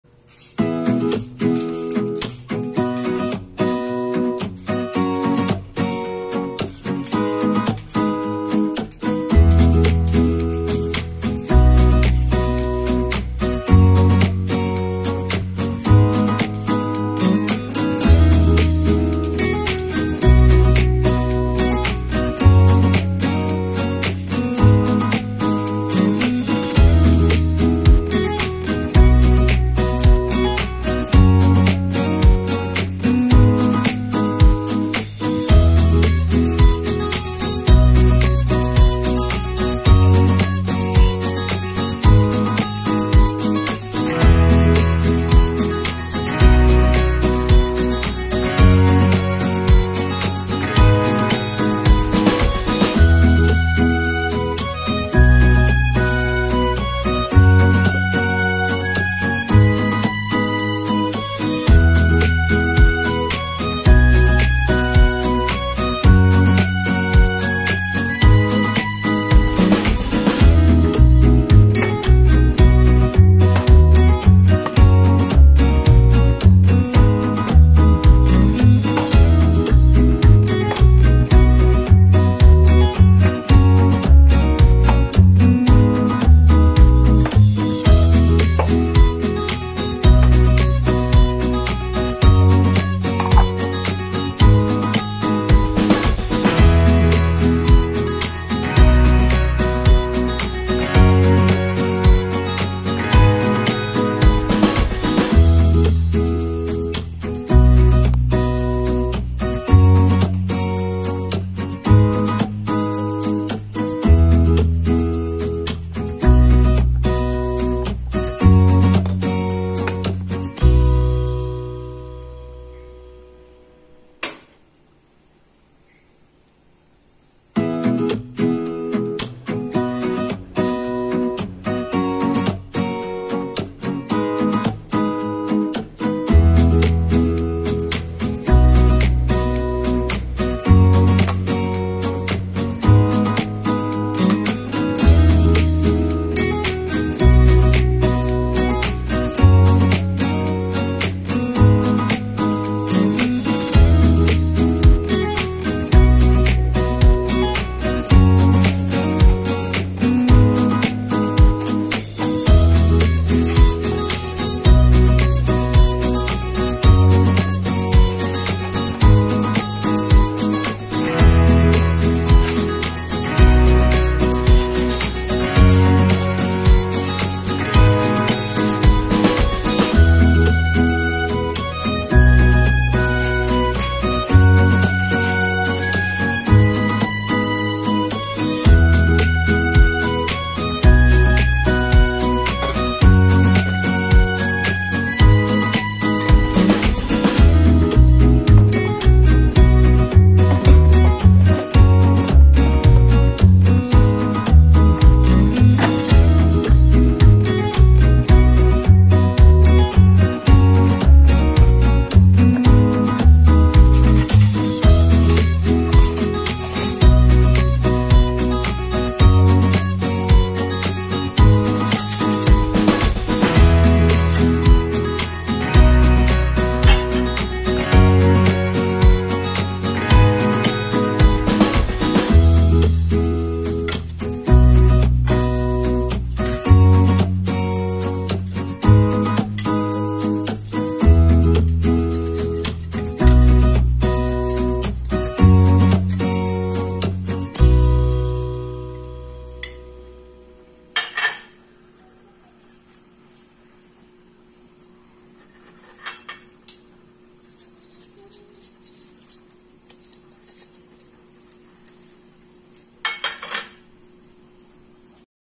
Perfect and crispy air fried roast potatoes
These Air fryer potatoes are suuuuuper easy and delicious!

Please click on subtitles for instructions :)